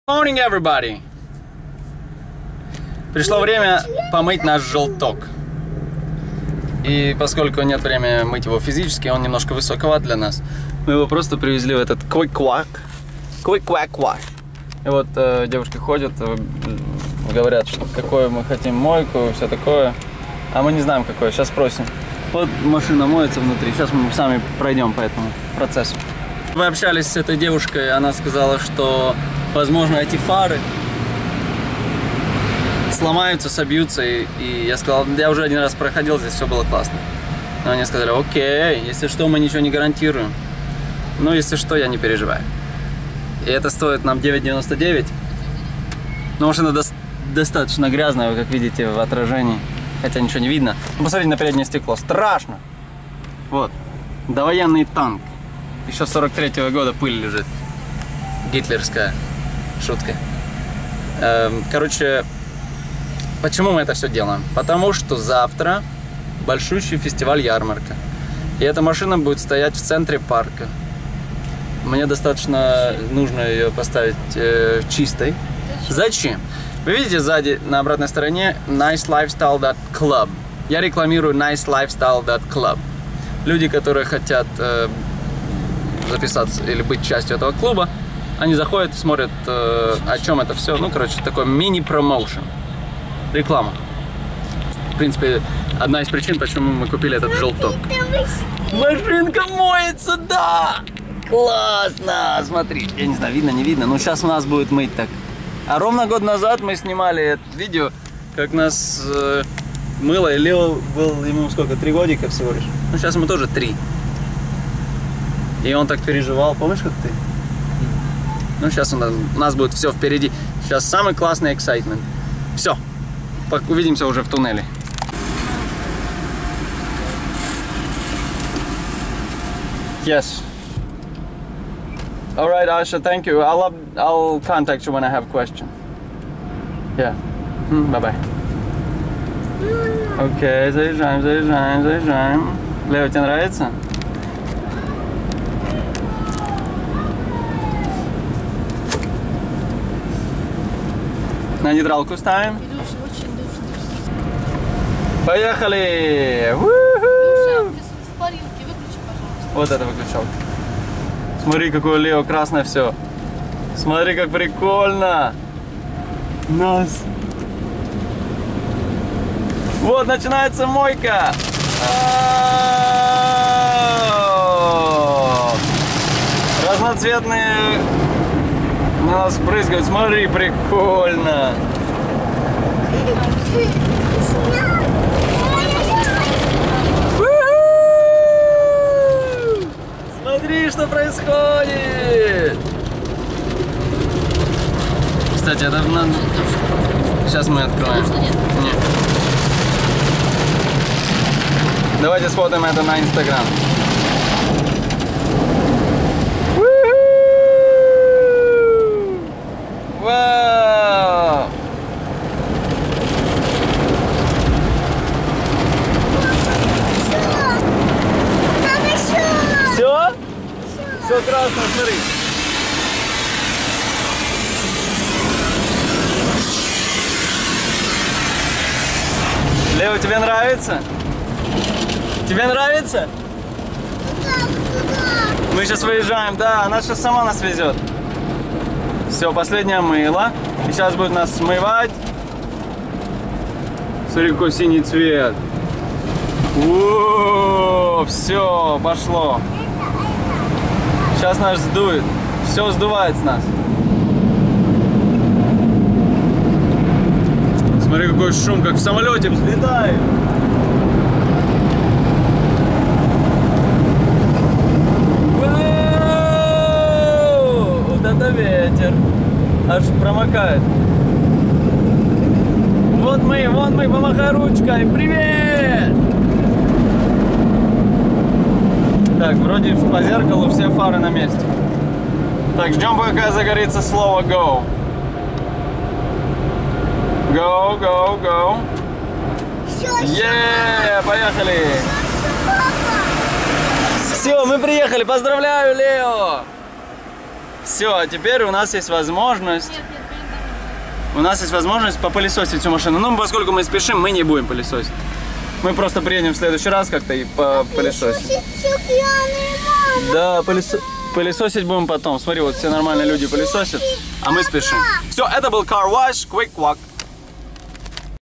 Мойка Желтка (Hummer H2)
Подготовка нашего желтка к выставке на Ярмарке. (реклама)

Yellow Hummer H2